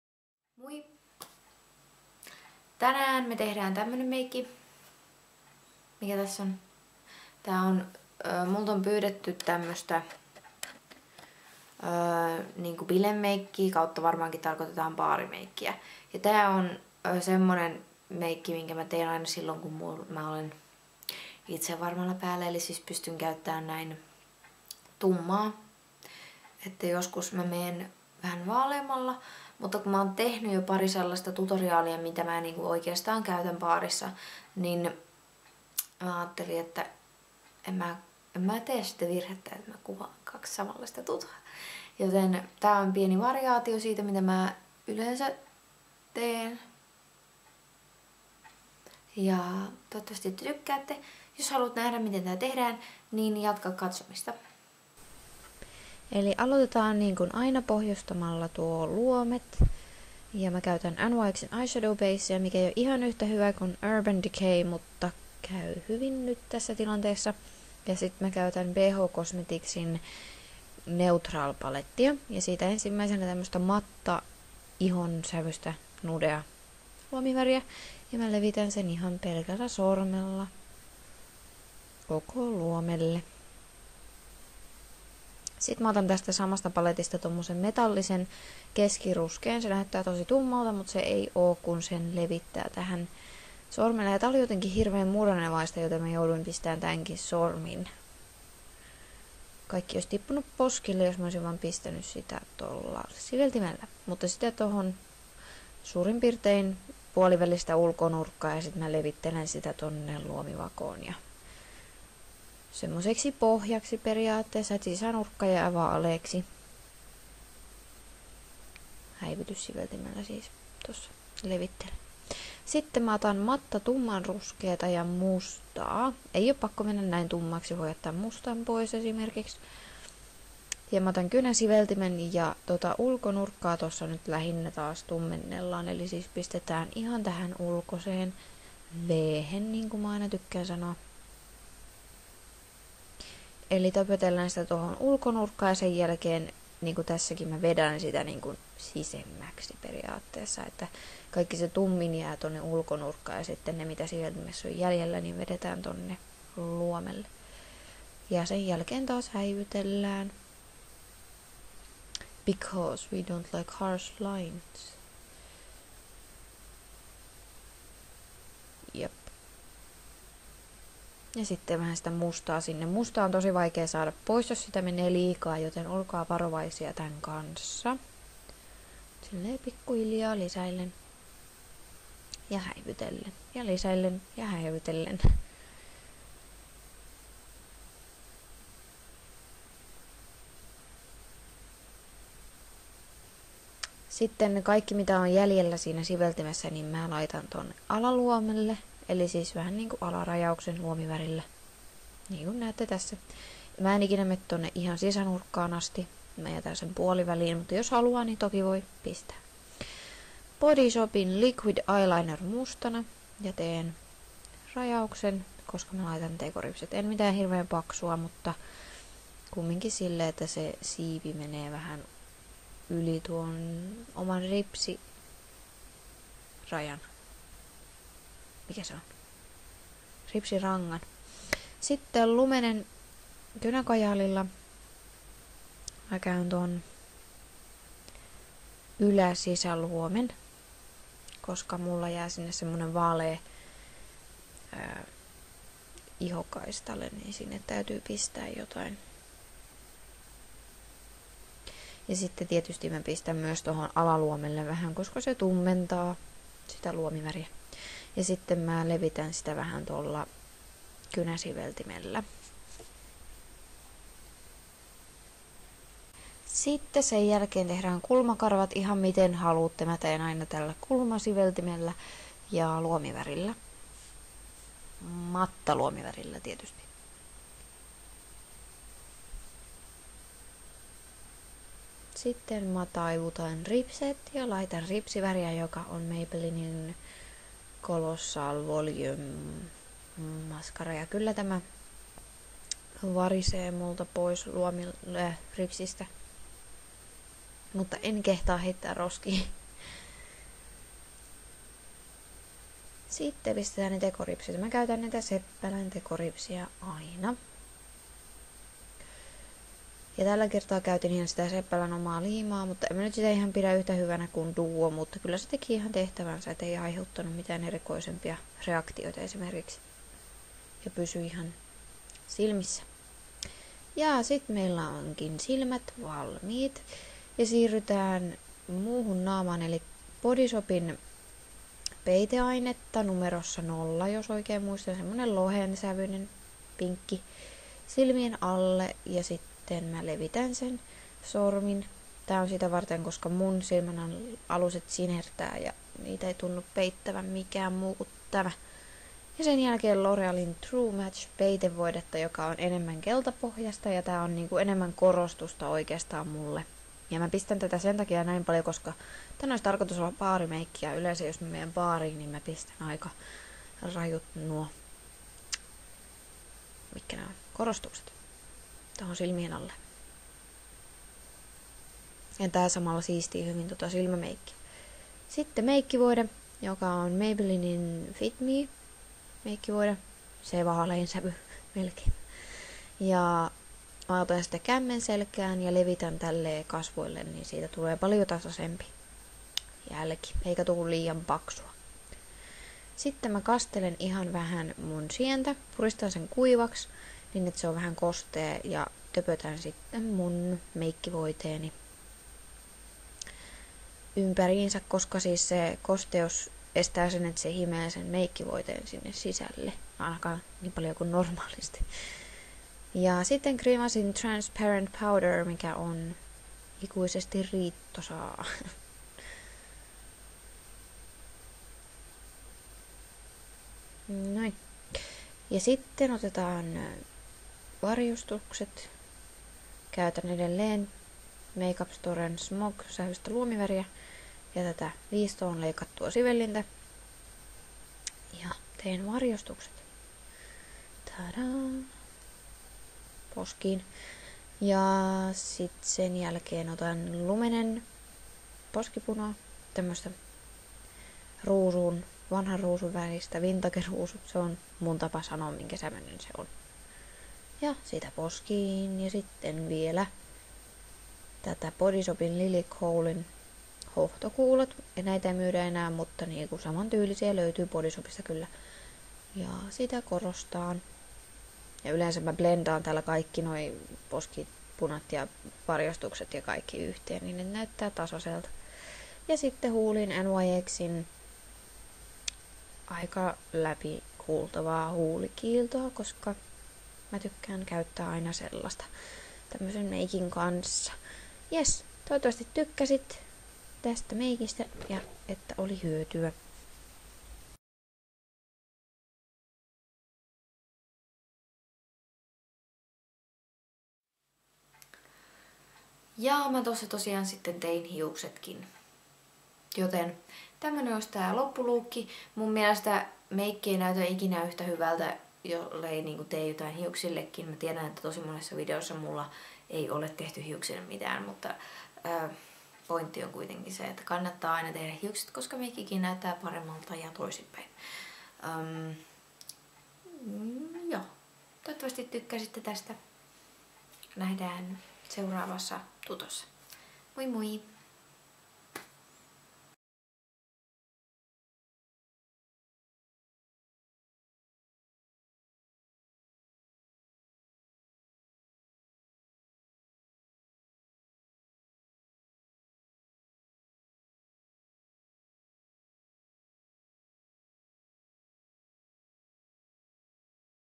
Tumma Ruskea SmokeyEye Baarimeikki | MaanoMeikkaa
KASVOT/FACE:
Meikkivoide: Fit Me
Puuteri: GrimasTransparent powder
Peiteaineet silmien alle: TheBodyShop peitevoide 0,Loreal True Match ivory

SILMÄT/EYES
Luomien pohjustus: NYX eyeshadow base
Luomivärit: BH Cosmetics: 88 Natural palette, Ben Nye Essentital Eye Shadow Palette
Ripsiväri: Maybelline Colossal Volume Mascara

KULMAT/EYEBROWS
Ben Nye Essentital Eye Shadow Palette

RAJAUKSET/LINERS
Lumene,
TheBodyShop liquid eye liner, musta

HUULET/LIPS
Huulikiilto: NYX Sugarplum

POSKET/CHEEKS
Poskipuna:Lumene, Touch of radiance, Charming Blush/Lumous 3
Varjostus: Make Up Store, Microshadow, smog

EXTRA
Tekoripset: Seppälä

SIVELTIMET/BRUSHES
Sigma
❥ I N   C O - O P E RA T I O N (Sponsored my YouTube partnership): BuzzMyVideos.com
❥ E D I T I N G : Windows Live Movie Maker (elokuvatyökalu)